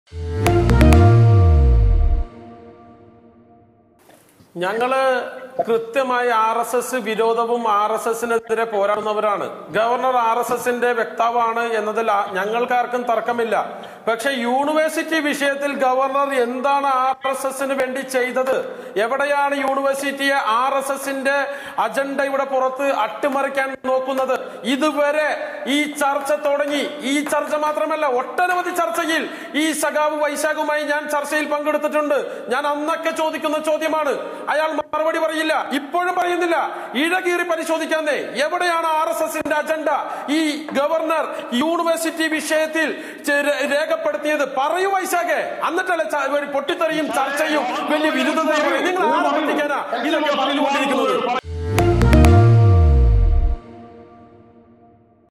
പറയൂ വൈശാഖെ, ഇഴകീറി പരിശോധിക്കാന്നെ|Questionhour|Youtalk
യൂണിവേഴ്‌സിറ്റി വിഷയത്തില്‍ ആര്‍എസ്എസിന്റെ അജണ്ട എവിടെയാണ് ഗവര്‍ണര്‍ നടപ്പാക്കിയതെന്ന് ജിന്‍ഷാദ് ജിന്നാസ്